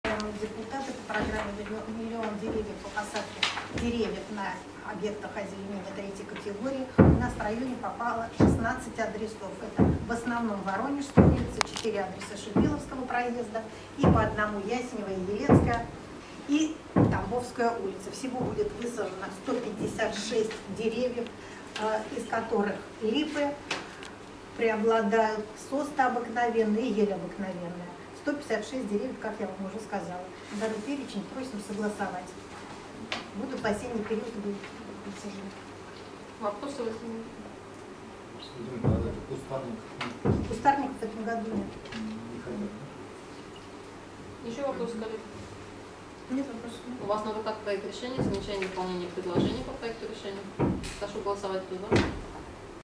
Березы, липы, рябины и другие деревья высадят в районе Орехово-Борисово Южное
Во время очередного заседания депутаты Совета депутатов муниципального округа Орехово-Борисово Южное приняли положительное решение по вопросу проведения работ по озеленению. Представители органов местного самоуправления утвердили адресный перечень и список растений, которые высадят уже этой осенью. Работы пройдут на 16 дворовых территориях.